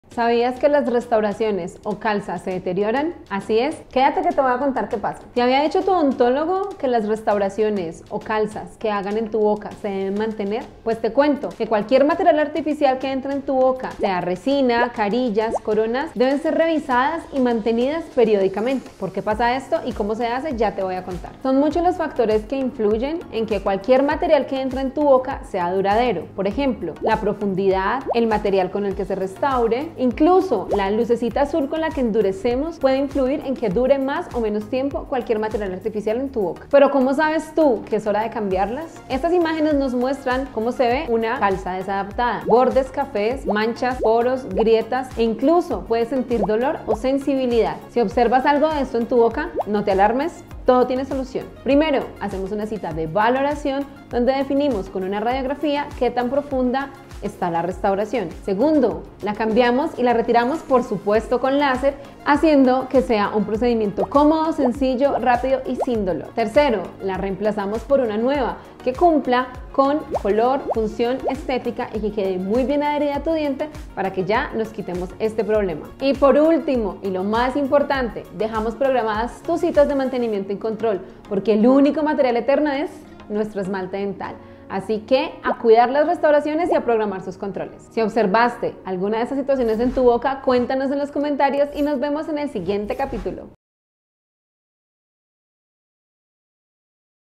¿Cuando se cambia una calza? ¿Como puedo saber si tengo mal una calza?🤔 - Odontología láser
Familia, en este video les explicamos todo sobre
🤗 ¡Esperamos les guste y sirva! ✅